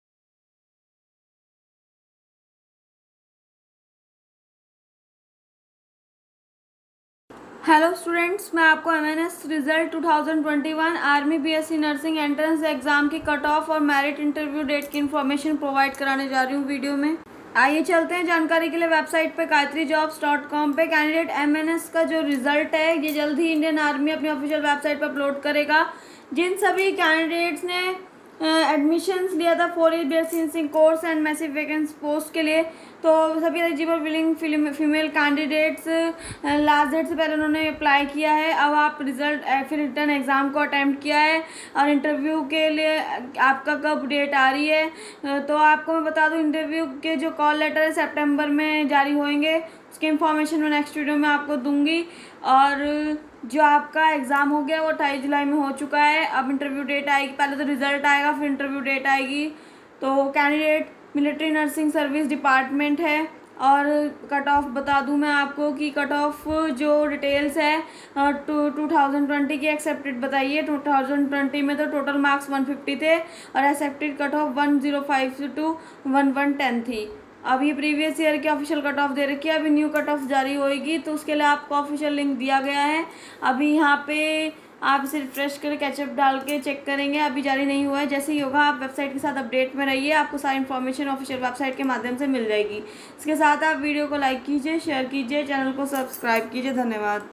MNS Result 2021 Download Indian Army B.SC Nursing Entrance Exam Cut Off Merit List Interview Date
The Indian Army has declared the MNS Result 2021 of the written examination/Interview for the applied applicants who want to take admission into 04 years B.Sc Nursing Course.
The last date to apply online for B.Sc Nursing Course was10th March 2021
Written Examination Date- 28th July 2021
MNS Written Exam Admit Card- 15th July 2021
MNS Written Exam Result - August 2021
MNS Interview Call Letter- September 2021
Check & Download Indian Army B.Sc Nursing Admission Exam Result by visiting the official website of the department or directly through the official link available